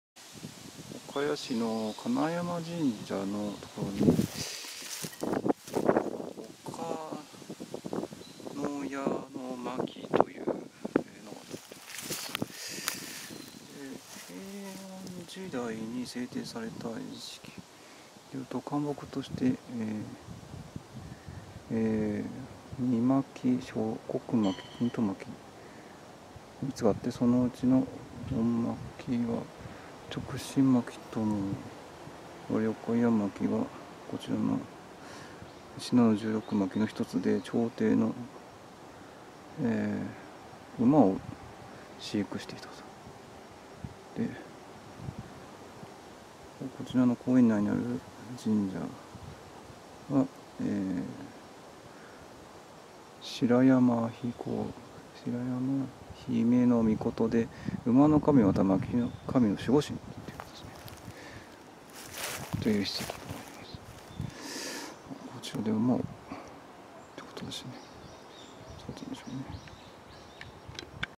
岡屋の牧 金山神社 小部澤社
岡谷市山手町1にある岡谷の牧　金山神社と小部澤社です

平安時代ここに岡谷牧という官牧があり、朝廷の御用馬を飼育したそうです。
小部澤社と金山神社がありますが、小部澤社が場所を貸していると
記載しているHPがありました。
白山姫命を祀っていて、馬の神、又は牧の守護神との事